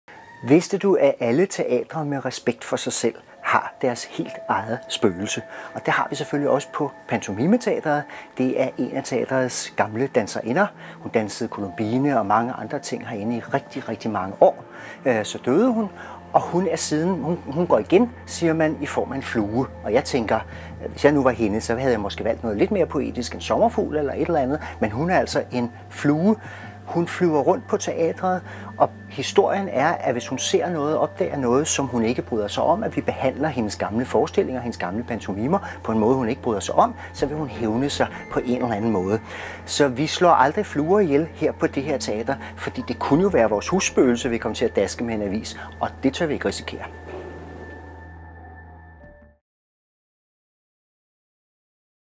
Spøgelse hjemsøger Pantomimeteatret
Vidste du, at Pantomimeteatret er hjemsøgt af et spøgelse? Én af teatrets gamle danserinder holder til i det gamle teater. Her kan du høre dramaturg og instruktør Henrik Lyding fortælle om spøgelset.